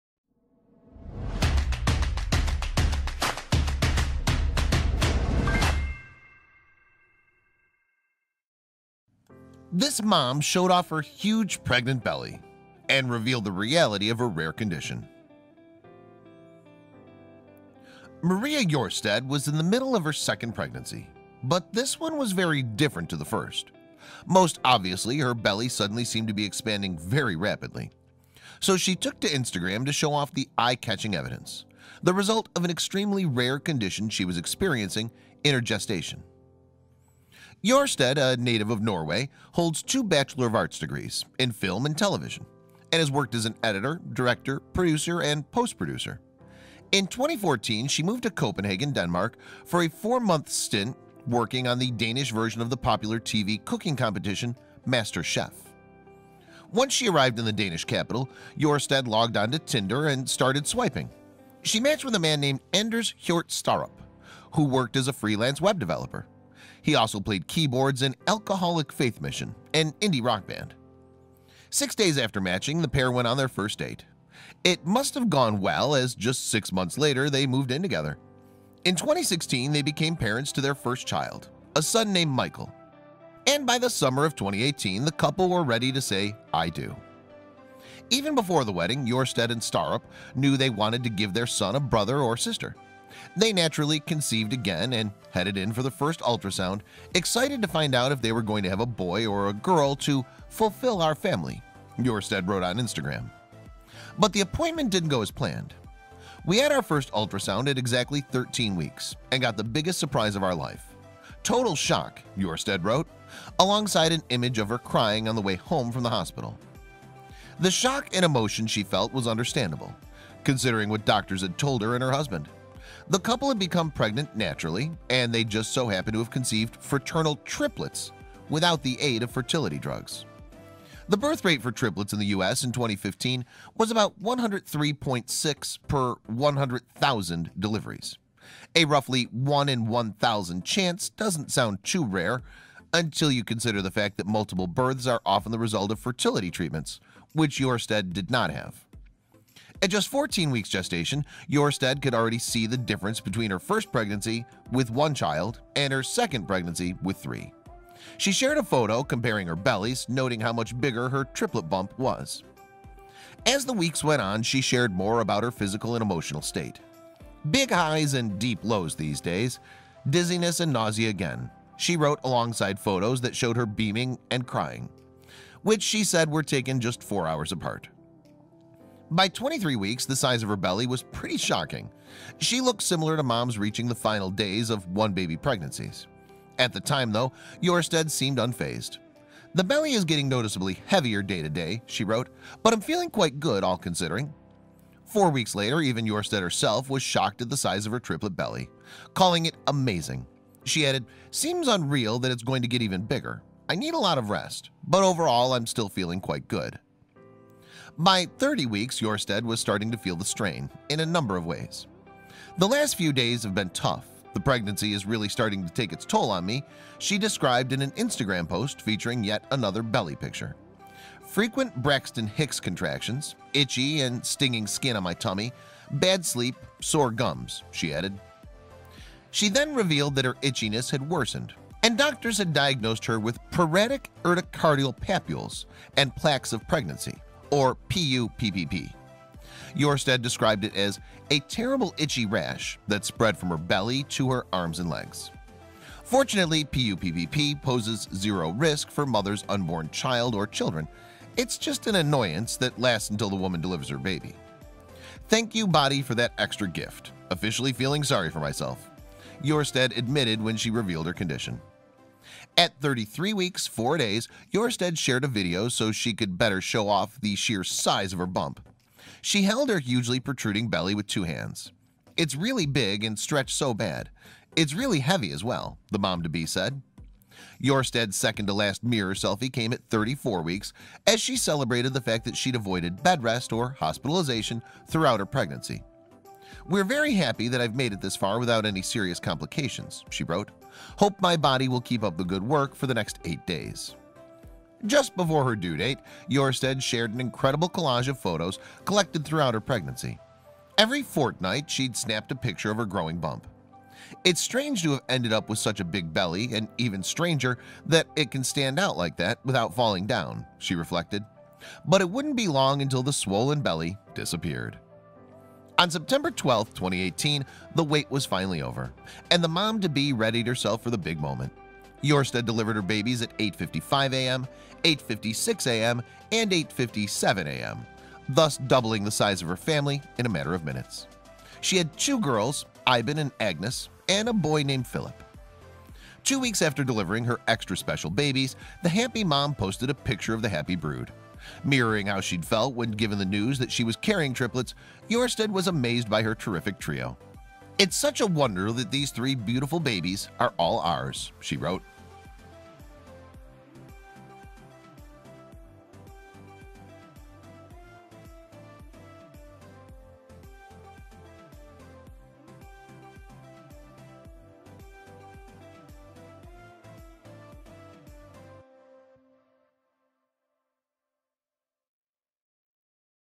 This Mom Showed Off Her Huge Pregnant Belly – And Revealed The Reality Of Her Rare Condition
Maria Jørstad and her husband wanted to give their young son a sibling. When she got pregnant, though, the ultrasound revealed how unusual her pregnancy was. So, she documented all of it on Instagram. ADVERTISEMENT Maria Jørstad was in the midst of her second pregnancy, but this one was very different to the first. Most…